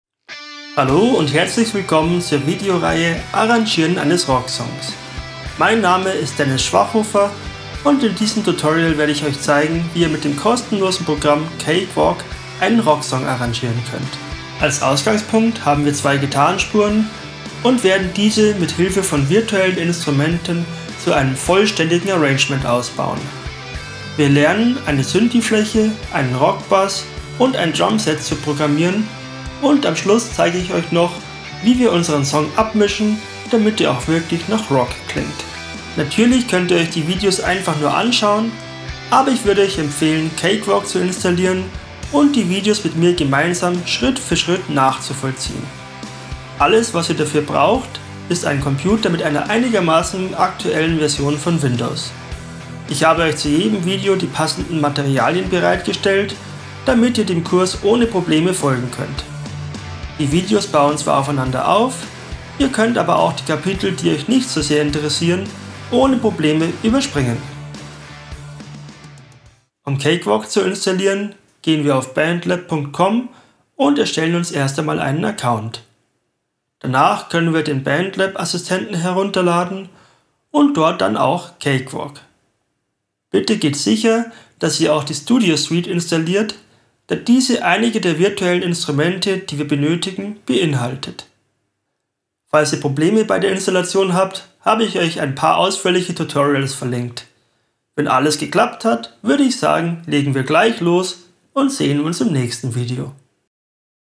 Produktion eines Rocksongs mit Cakewalk - 1. Intro
In dieser Videoreihe lernt ihr, wie ihr einen Rocksong mit Cakewalk produzieren könnt. Von der Leadgitarre über die Drums bis hin zum Mischen zeige ich euch alle relevanten Arbeitsschritte , um aus einer einfachen Idee einen richtigen Rocksong entstehen zu lassen.

♫ Dennis Schwachhofer composes music in various styles from happy to hard ♫